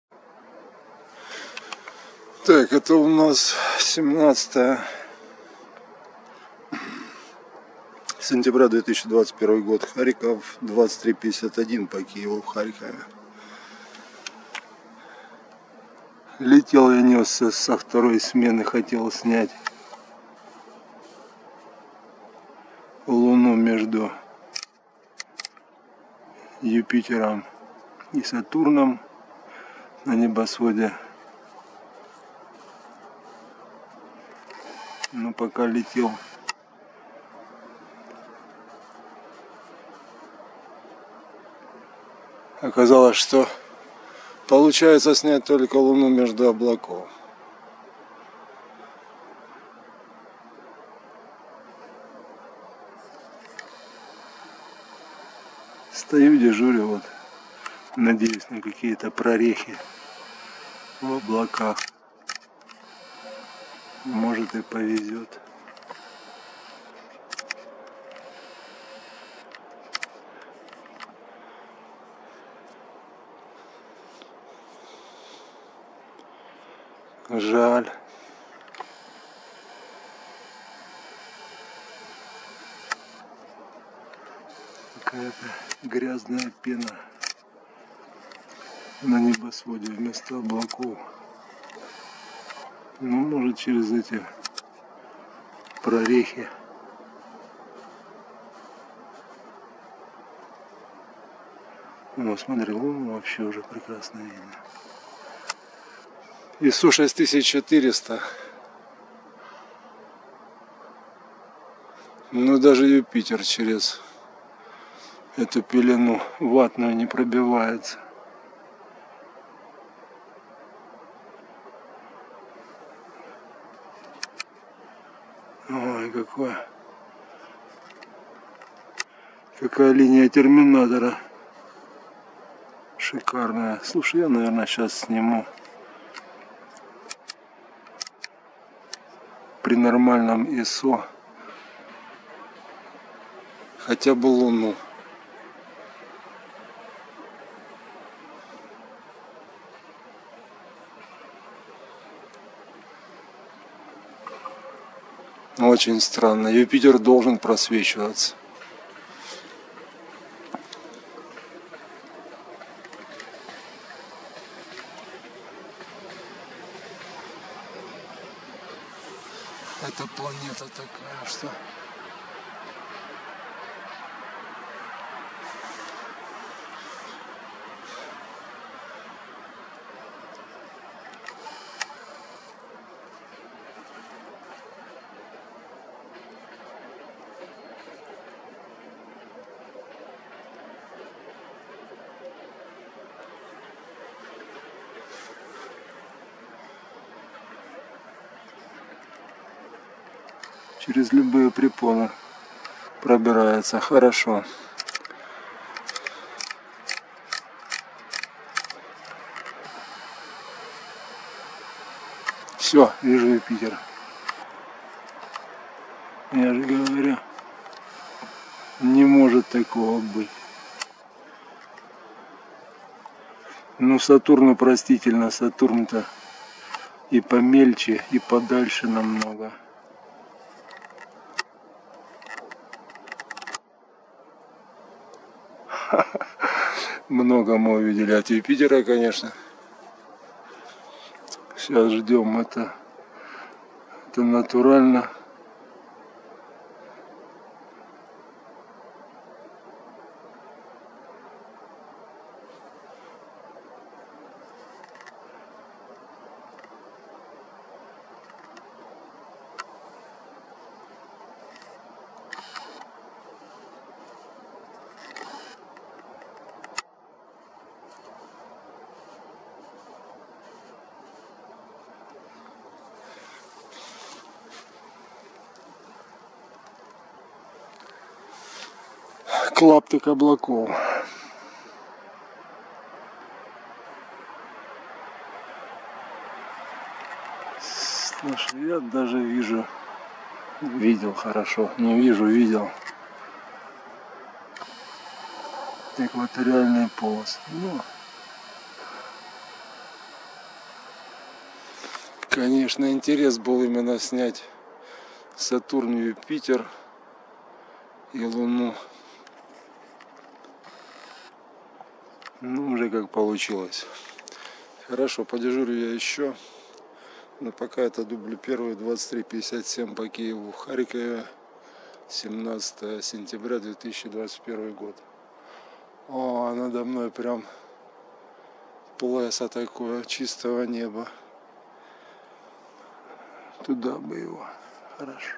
Луна и Юпитер 17 сентября 2021
Луна и Юпитер через облака 17 сентября 2021